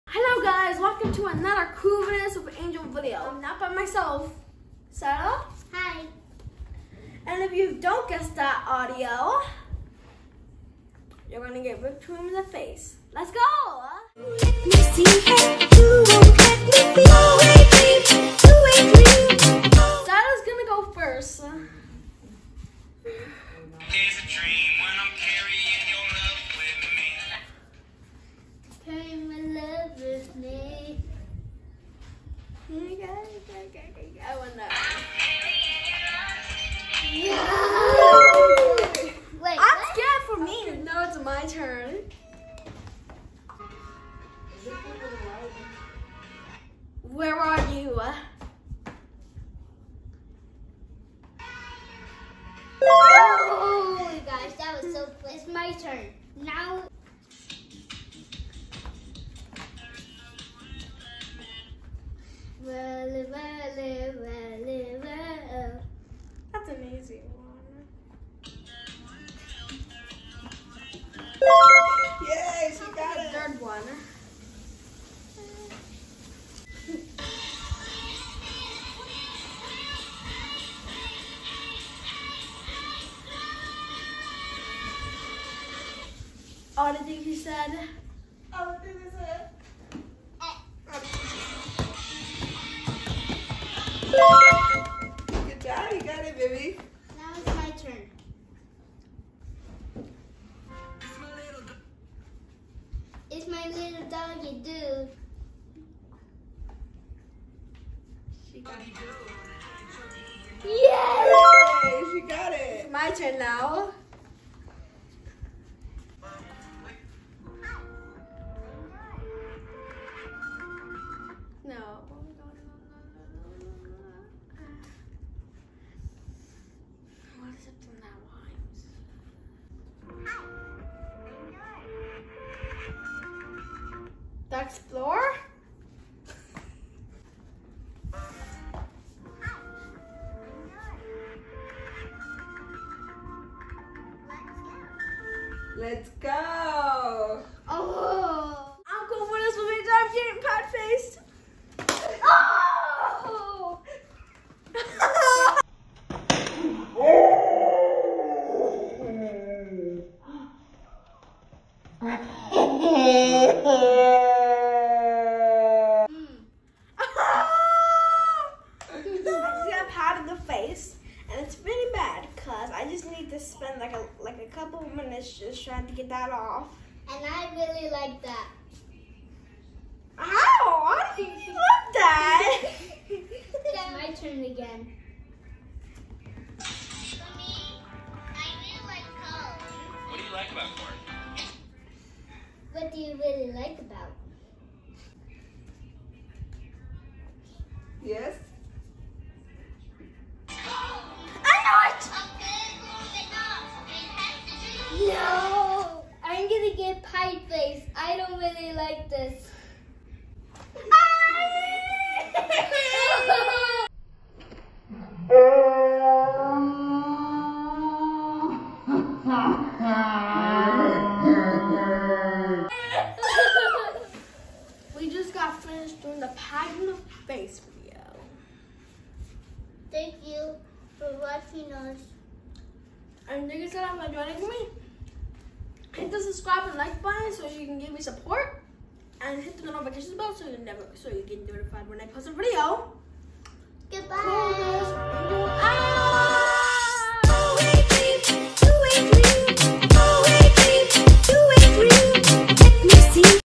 Finish that Tiktok sound or get Pie Face 😱 *extremely hilarious*
I had so much fun much filming this video with my friend! 💙❤️ You think you can finish that tiktok lyrics ? How do you think my friend Siela & I did ? 👦🏻👧🏻✨

~ Thankful for every one of you!
~ I love making content for you guys ✨